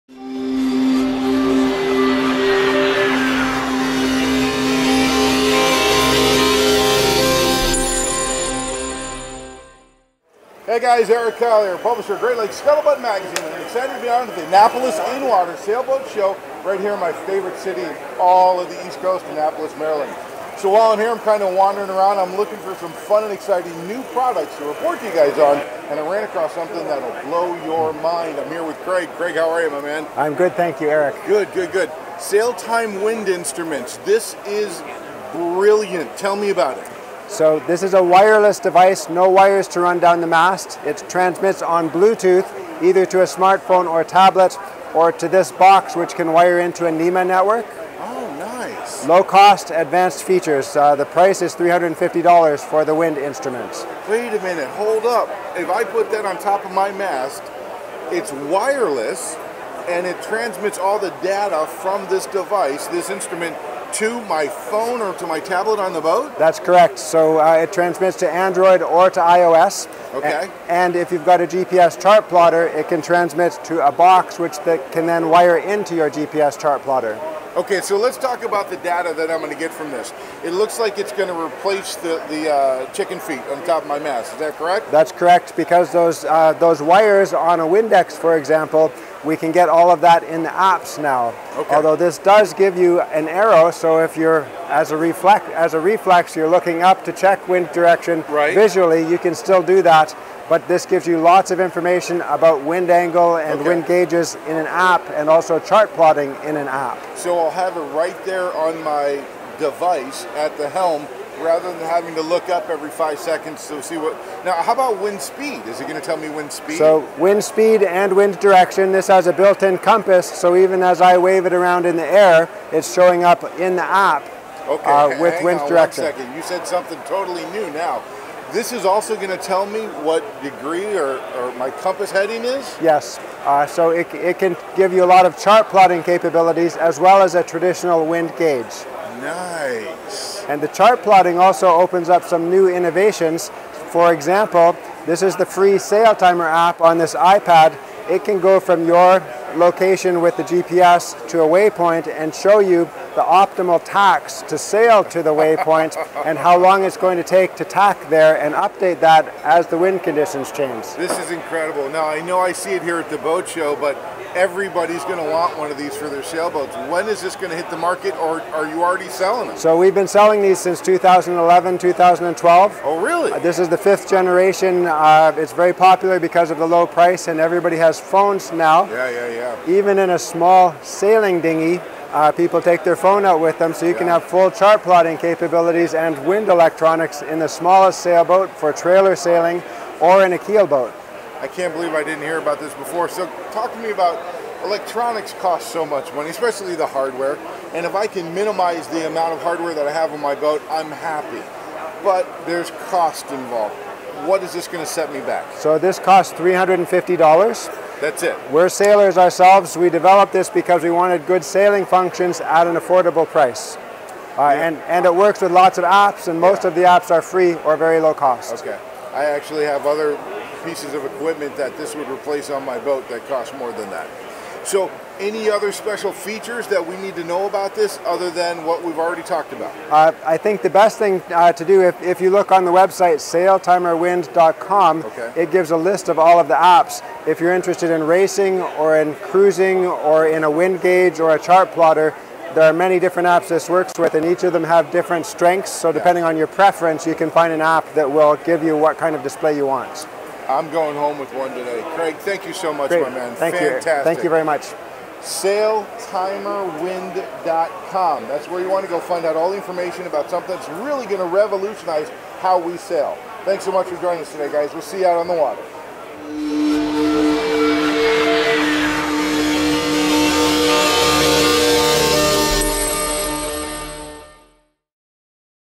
SailTimer Bluetooth Solar Powered Wind Instrument With Craig At The 2018 Annapolis Boat Show!
Wow!
We were blown away with this one!
Craig from SailTimer Wind Instruments introduced us to the next level of wind and direction information for us sailors!
The SailTimer will connect via Bluetooth to both iOS or Android, or even Air Link it to your NMEA GPS chart plotter! Track realtime air speed, direction, angle, GPS, and compass on your wireless device no matter what size boat you're on! Perfect for large vessels or even small sailboats looking for advanced features.
The SailTimer app is available on iOS and Android App Stores for free, and the device is $350.